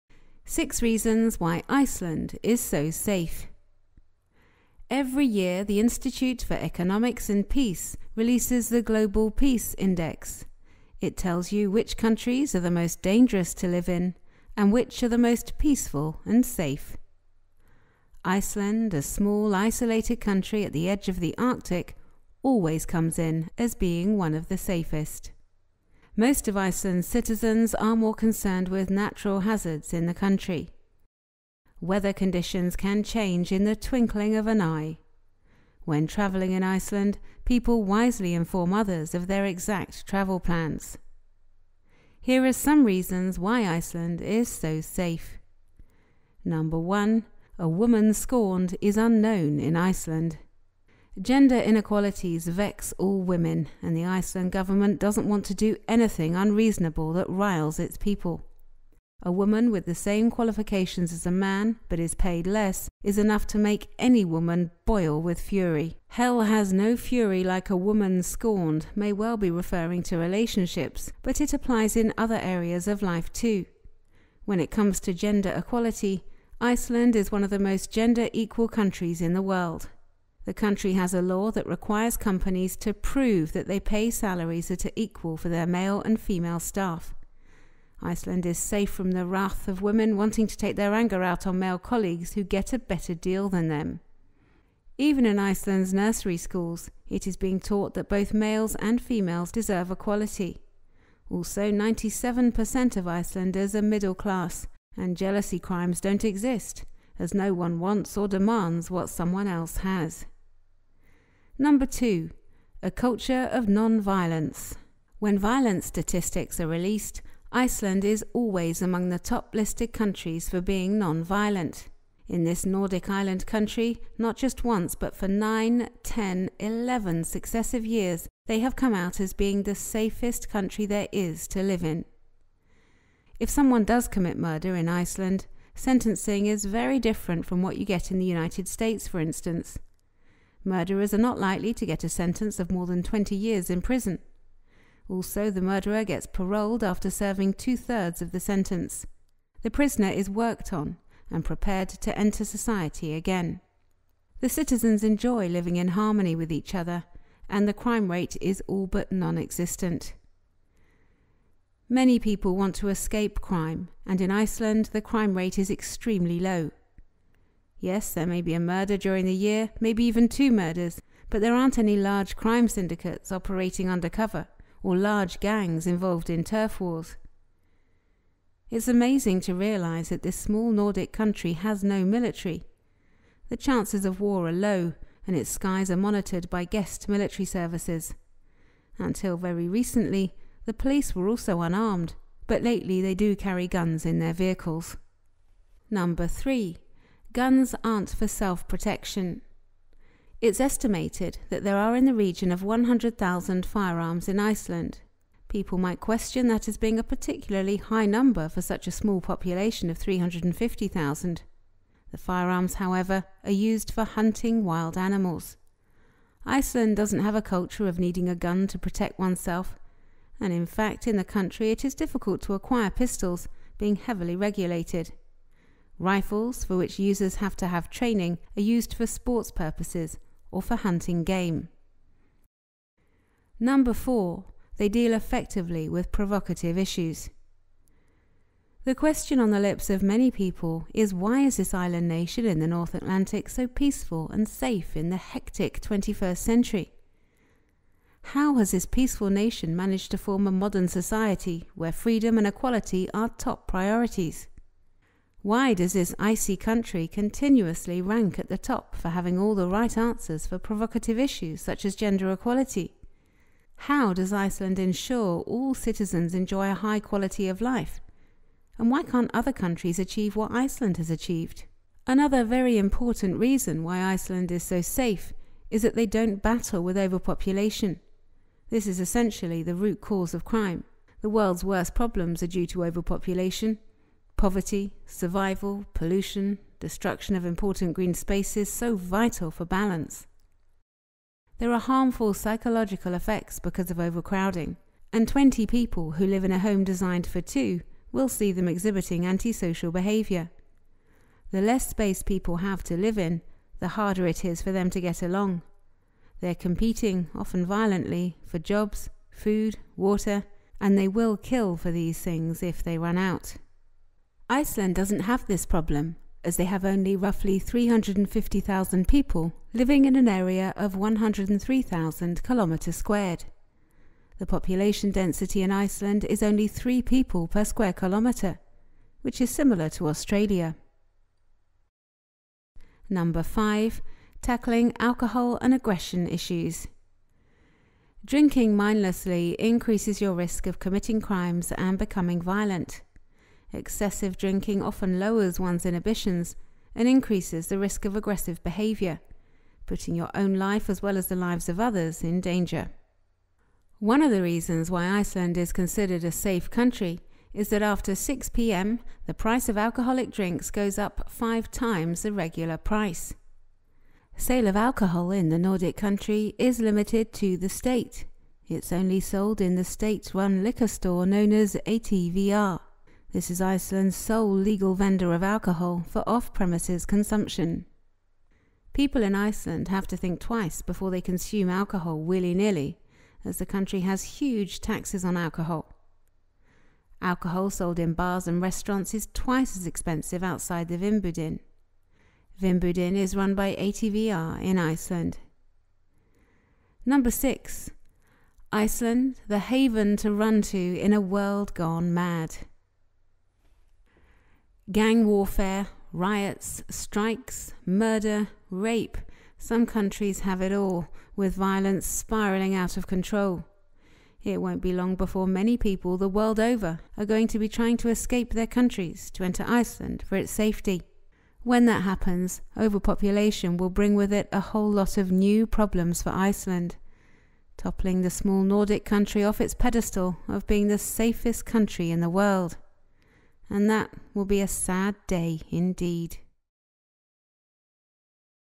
6 Reasons Why Iceland Is So Safe
Every year the Institute for Economics and Peace releases the Global Peace Index. It tells you which countries are the most dangerous to live in and which are the most peaceful and safe.

Iceland, a small, isolated country at the edge of the Arctic, always comes in as being one of the safest.

Most of Iceland’s citizens are more concerned with natural hazards in the country. Weather conditions can change in the twinkling of an eye. When traveling in Iceland, people wisely inform others of their exact travel plans.

Here are some reasons why Iceland is so safe.